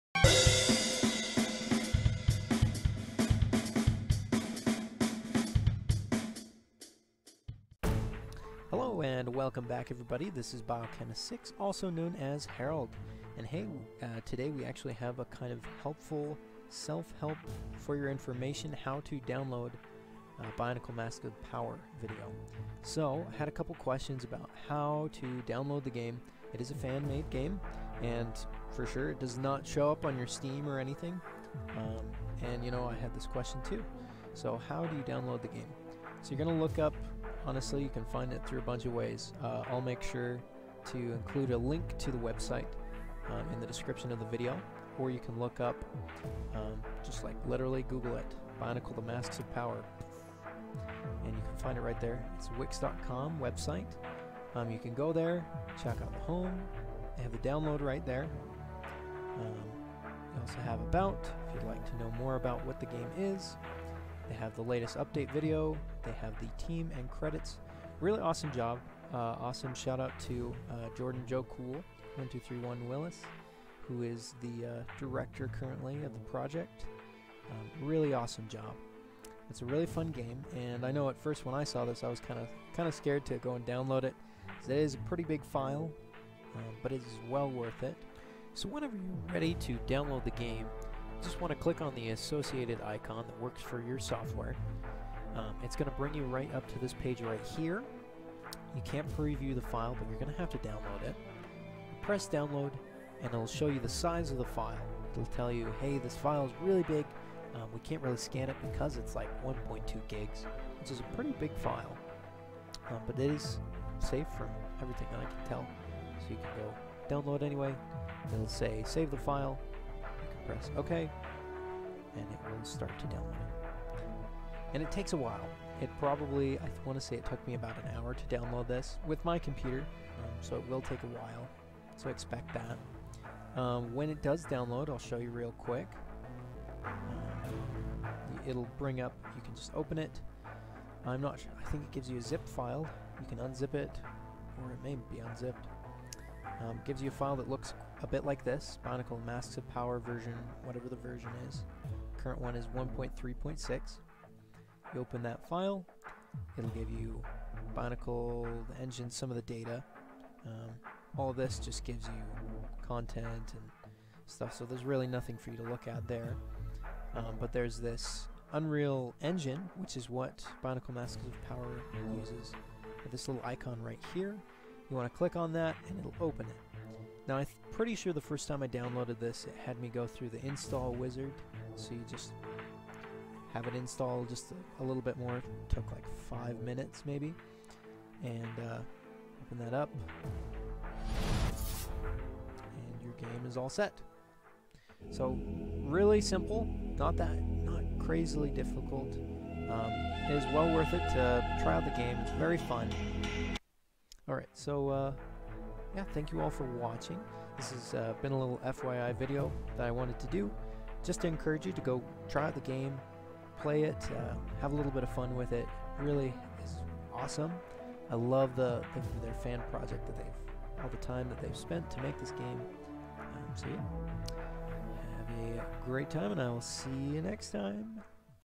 How To download Bionicle: the Masks of Power
Here's a quick how to video for downloading Bionicle: the Masks of Power (the game).  Sorry the sound is so quiet.